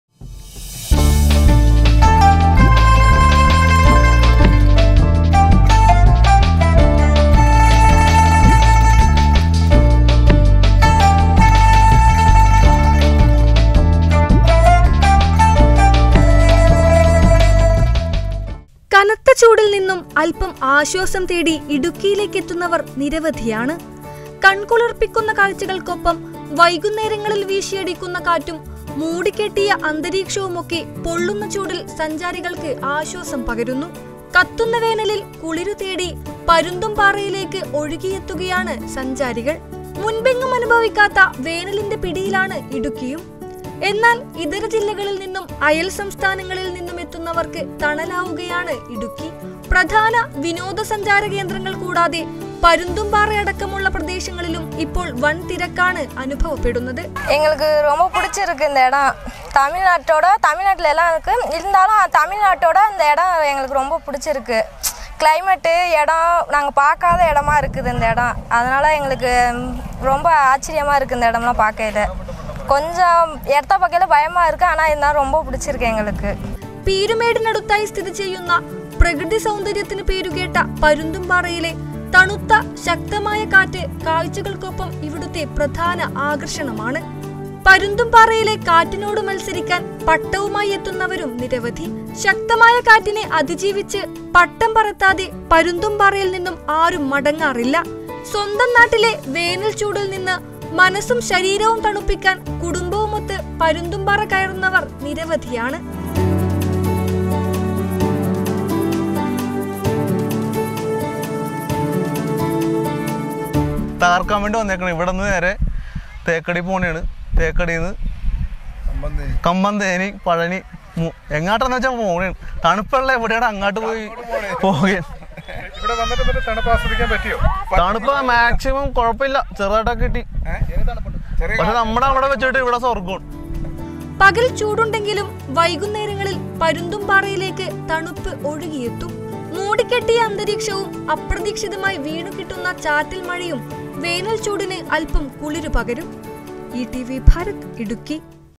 വേനൽച്ചൂടിൽ അൽപം ആശ്വാസം, പട്ടവും പറത്താം
കനത്ത ചൂടില്‍ നിന്ന് അൽപം ആശ്വാസം തേടി ഇടുക്കിയിലേക്ക് എത്തുന്നവര്‍ നിരവധിയാണ്. കൺകുളിർപ്പിക്കുന്ന കാഴ്‌ചകള്‍ക്കൊപ്പം വൈകുന്നേരങ്ങളില്‍ വീശിയടിക്കുന്ന കാറ്റും മൂടികെട്ടിയ അന്തരീക്ഷവുമൊക്കെ പൊള്ളുന്ന ചൂടിൽ സഞ്ചാരികള്‍ക്ക് ആശ്വാസം പകരുന്നു. ചൂടിൽ നിന്നും ആശ്വാസം തേടി ഇടുക്കിയിലെ പരുന്തുംപാറയിലേക്ക് ഒഴുകിയെത്തുകയാണ് സഞ്ചാരികൾ.